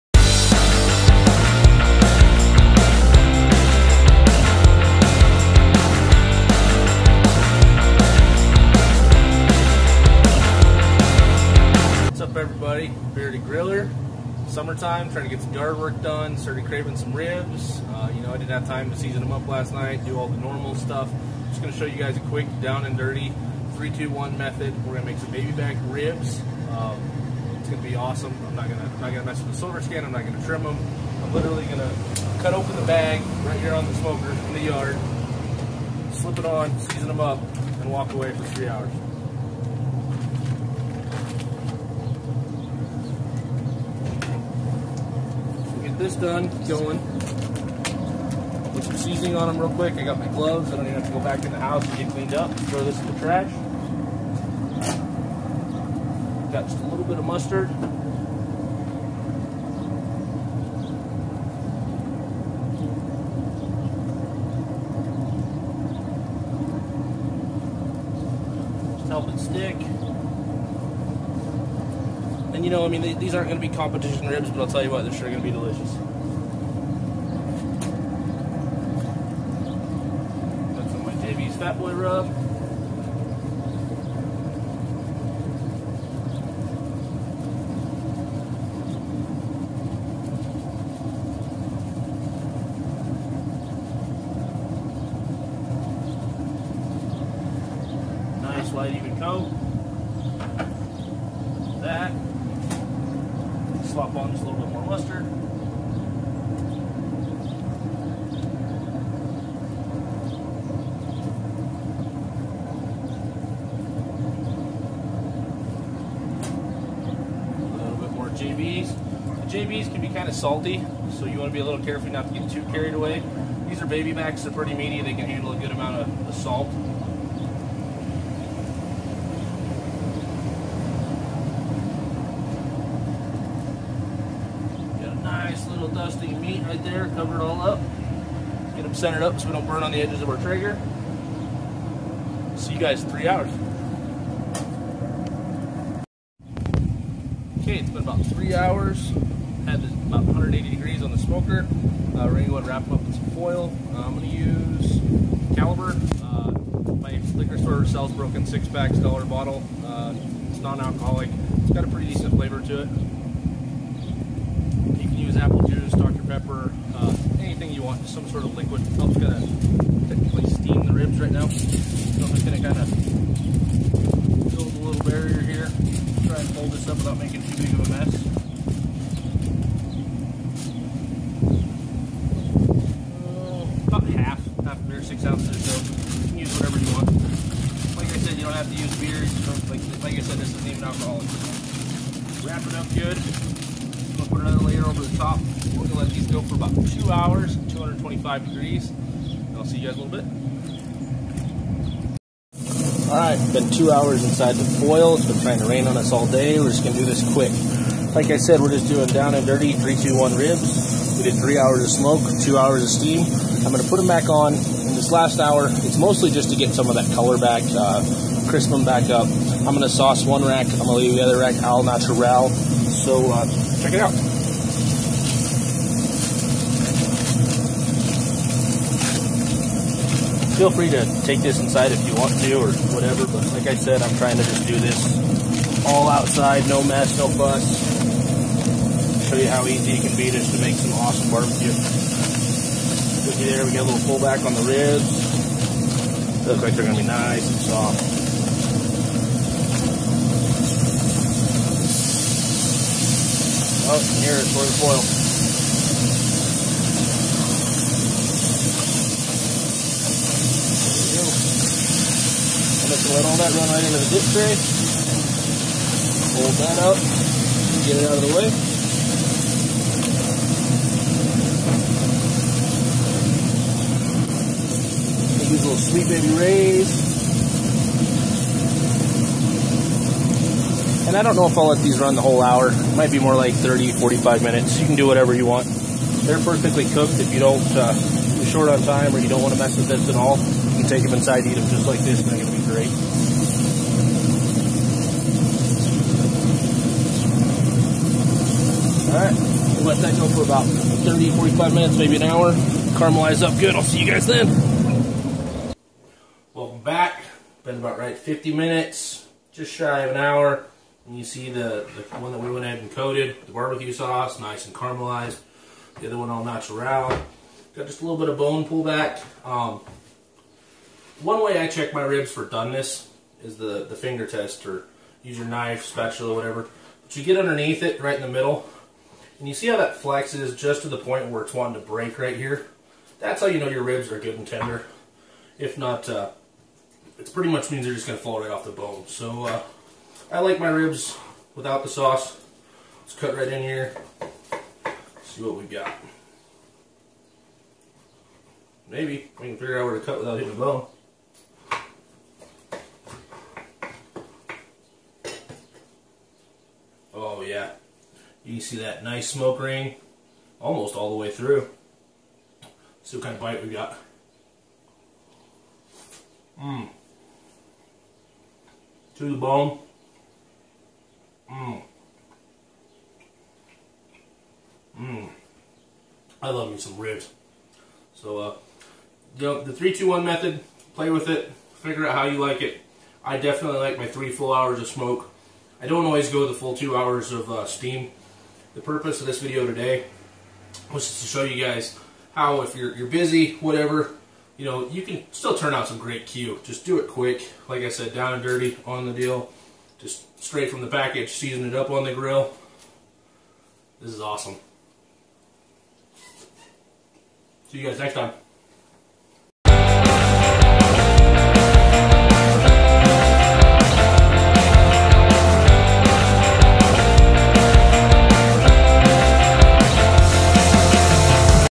3-2-1 baby back ribs on the traeger!
In this video I make some super easy baby back 3-2-1 style

I try to do this recipe is easy as possible and show you it doesn't take a lot of effort to make some really good ribs. I start by rubbing the ribs with mustard just to help the rub stick and don't worry if you don't like mustard you won't be able to taste it when it's done cooking.

 I put the traeger on the smoke setting which is usually about 180 degrees for 3 hours and walk away!

After your 3 hours is up wrap your ribs tightly in foil with about a cup juice or beer or anything else you might braise something in. We're going to bring our temperature up to the 225 degrees and let cook for another 2 hours depending on how soft you like your ribs. This step I will play with sometimes, usually less time so the ribs don't completely fall off of the bone!

Now in the last hour remove the ribs from your foil, they're perfectly cooked and Ready-to-Eat at this point you're just trying to get some of that color and texture back from them steaming. If you like to sauce them now is the time so it can caramelize. You don't have to go the full hour if you're short on time or like the way they look take them off and eat!